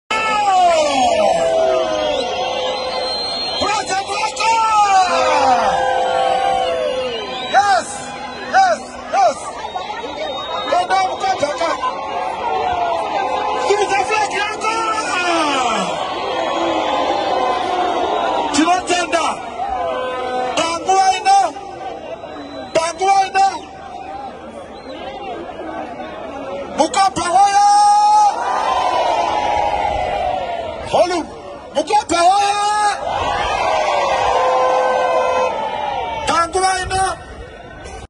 Chamisa paints Mkoba, Gweru yellow in mega CCC rally
People’s President Nelson Chamisa paints Mkoba, Gweru yellow in mega CCC rally